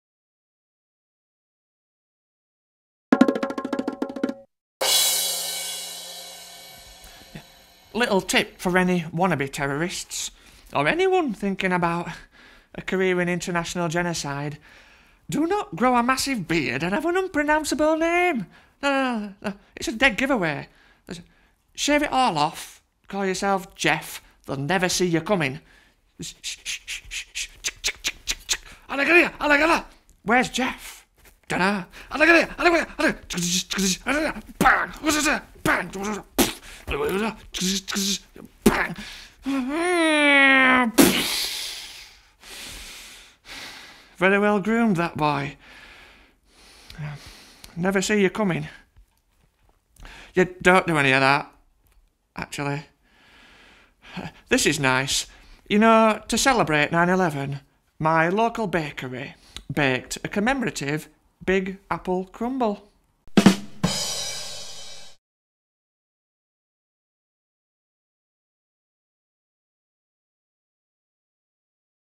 Tips For Wannabe Terrorists
Hello Tedsters .. feeling safe at home? Here are few handy tips from your Uncle Ted. Featuring the town's most eligible spatula.

Pro Cook Wide Fish Slice Spatula in Stainless Steel